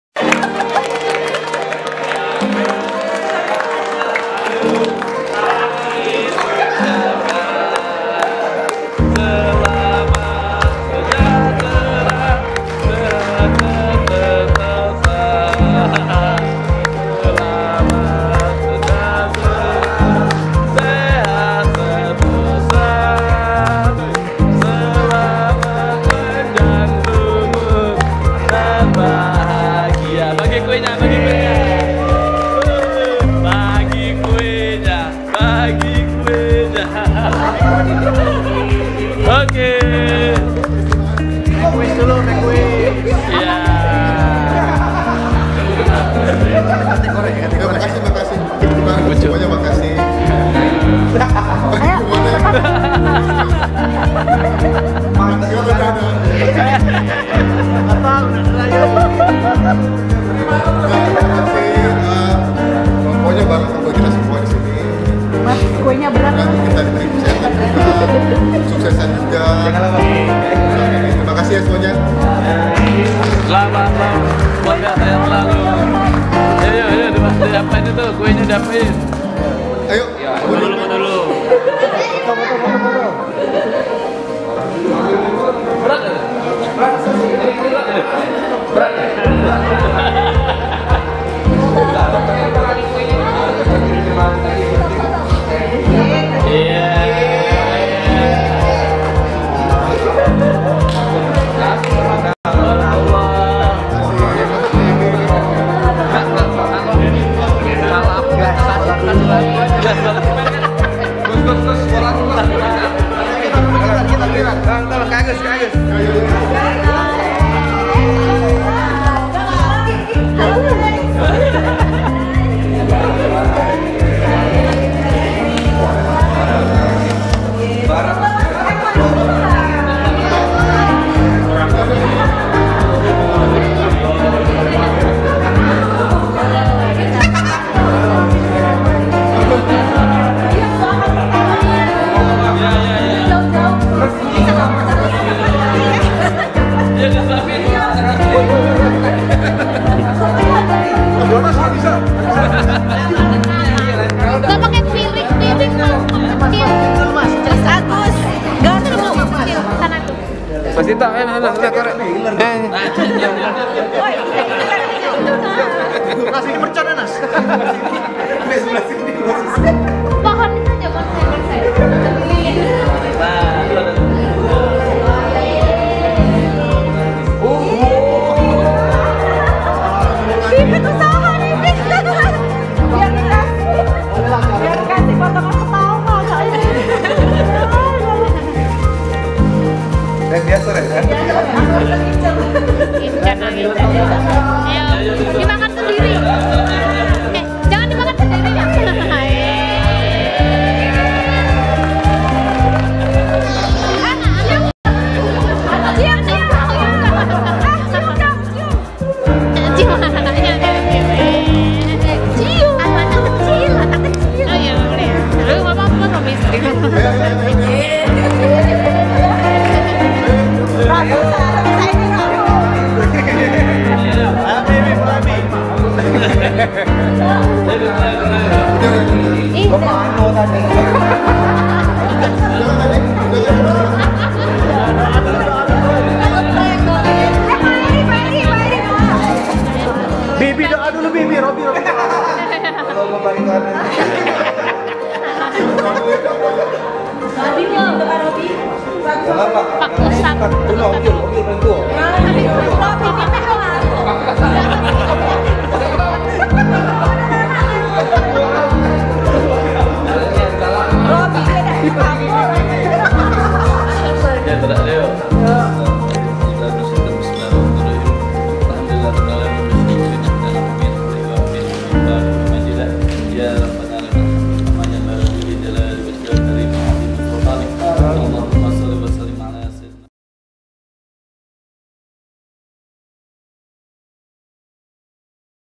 a Big Surprise!
Hari ini bener-bener dapat surprise dari my lovely wife and my lovely son. Nggak nyangka tiba-tiba ada di Kantor:) Thank you my lovely wife @triasther , my lovely son @keane_mikaeel, always love you both.

And thank you to @novitaambarsari2211 and @susyaprida for supporting
Semua temen dikantor yang telah mendo'akan, thank you banget!

Barakallah semuanya, Aamiin.